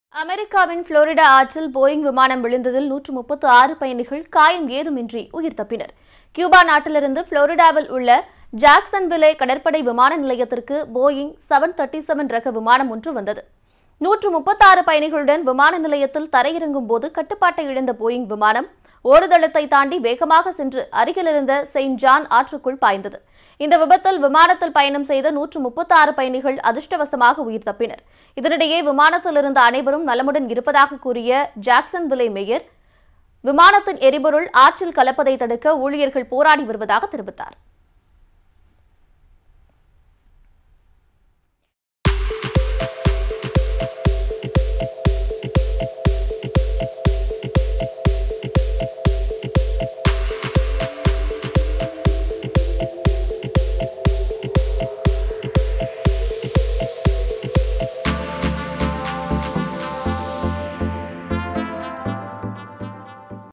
கட்டுப்பாட்டை இழந்ததால் ஆற்றுக்குள் விழுந்த விமானம் | அதிர்ஷ்டவசமாக உயிர் தப்பிய 136 பயணிகள்
அமெரிக்காவின் புளோரிடா ஆற்றில் போயிங் விமானம் விழுந்ததில் 136 பயணிகள் காயம் ஏதும் இன்றி உயிர் தப்பினர்